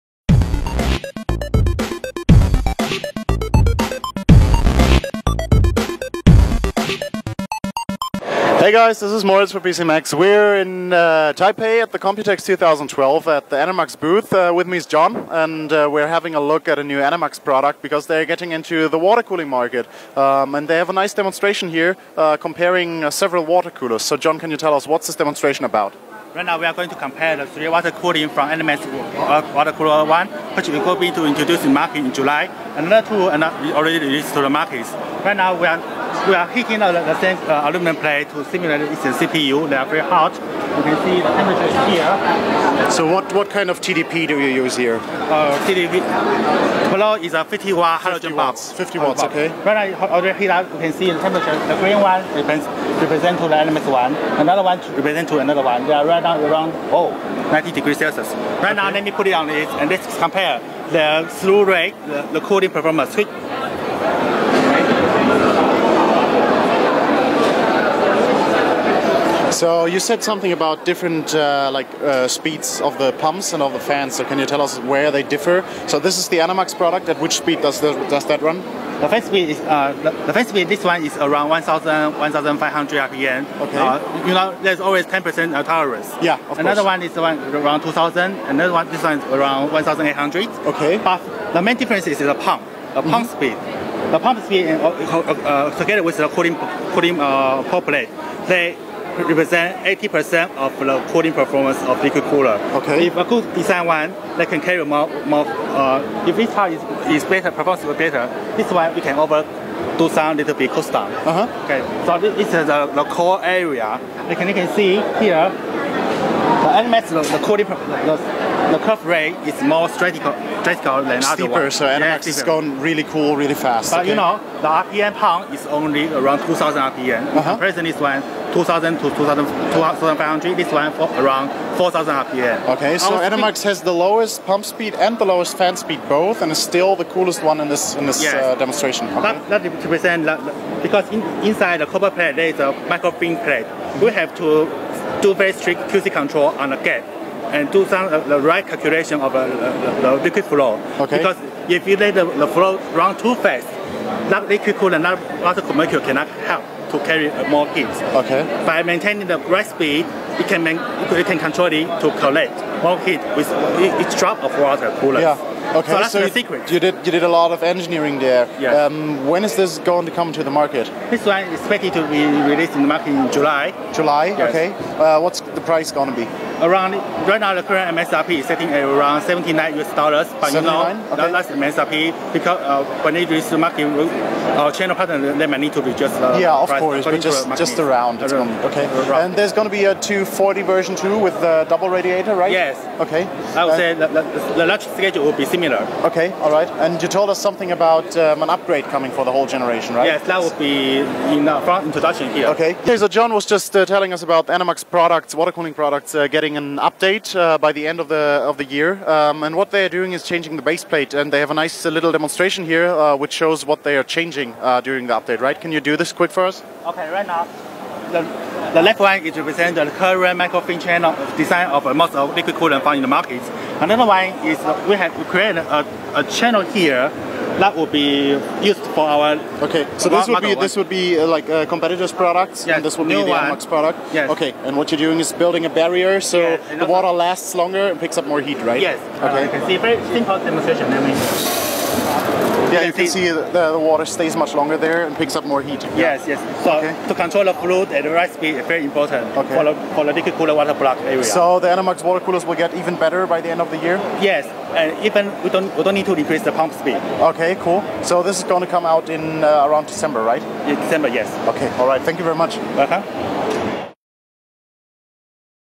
Computex 2012: Enermax zeigt fertige Kompakt-Wasserkühlung
Auf der Computex 2012 in Taipei zeigt Enermax zum ersten Mal die fertige Kompakt-Wasserkühlung ELC-120. Obwohl das Produkt vielen Konkurrenzprodukten ähnelt soll es deutlich leistungsstärker ausfallen.